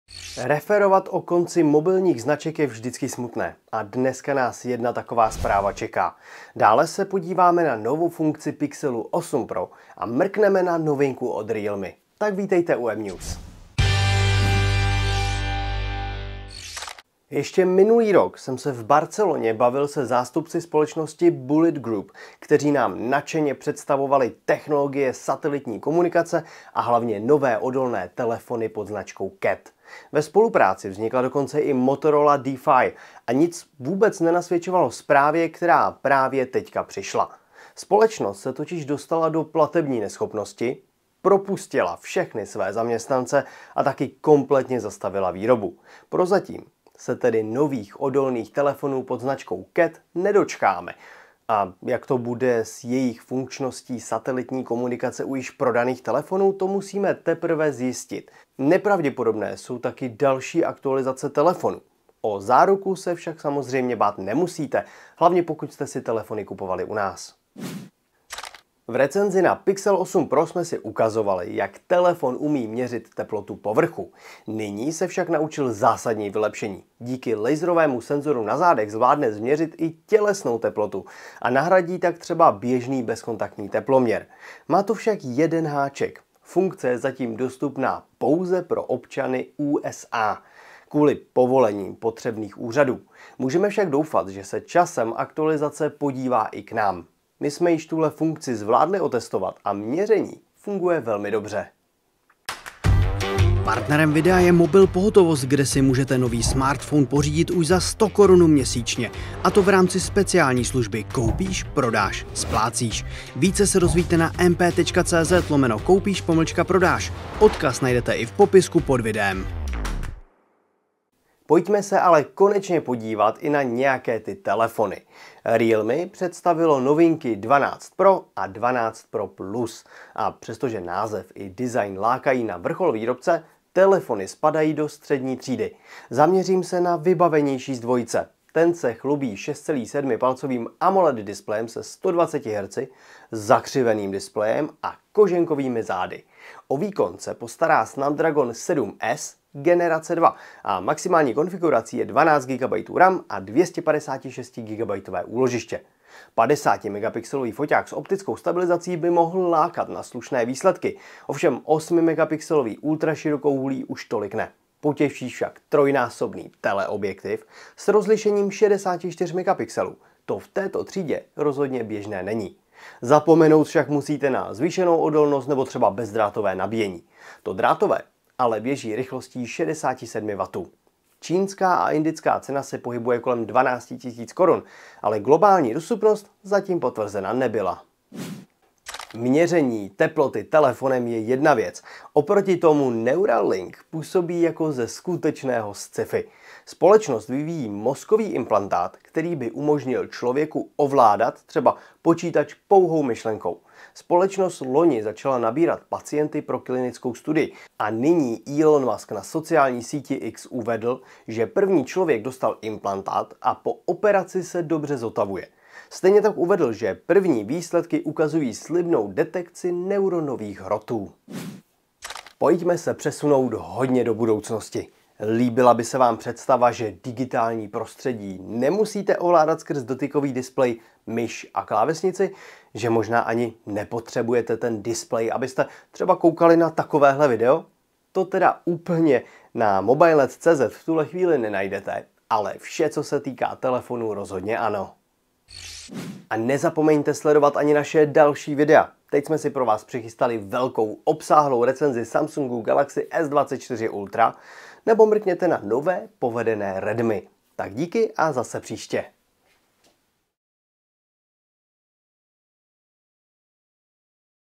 Výrobce telefonů CAT končí - mNews
Telefony značky CAT nyní už jen tak nespatříme, jejich výrobce končí
Pixel 8 Pro se naučil novou vychytávku, Realme 12 Pro lahodí oku a Musk posílá pozitivní zprávy ohledně mozkového implantátu
mNews přináší výběr těch nejzásadnějších zpráv ze světa technologií